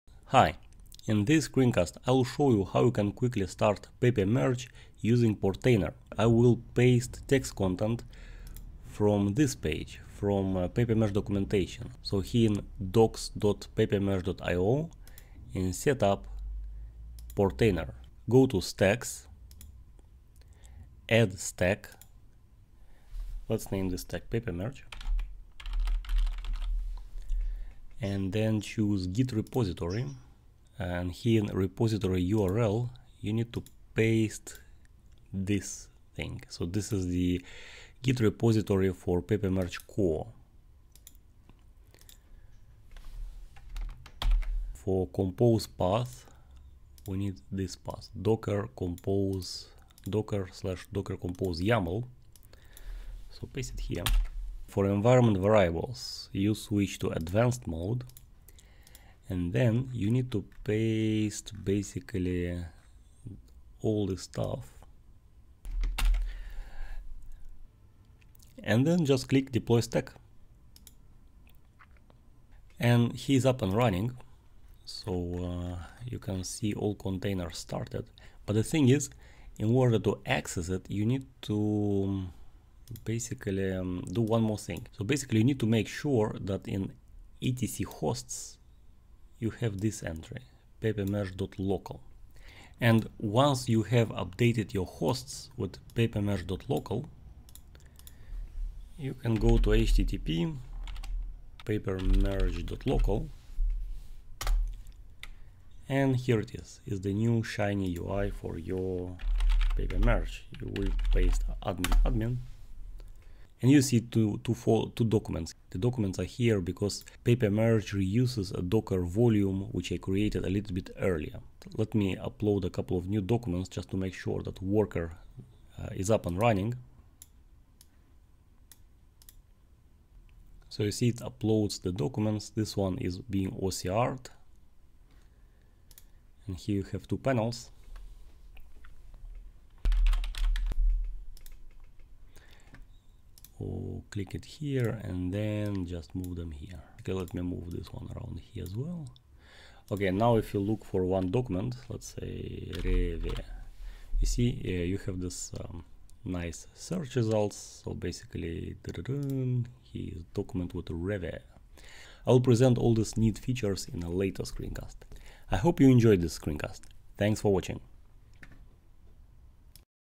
Start Papermerge in 2 minutes with Portainer
In this screencast I will show how you can start on your local computer latest version of Papermerge in just 2 minutes with Portainer.

In screencast I paste text (like urls and variables) from this documentation page.

Please keep in mind that as of recording this video latest version (2.1) of Papermerge is still in alpha - this means that it is feature incomplete and has lots of bugs - don't use alpha version in production environment. Alpha versions are meant only for demo purposes.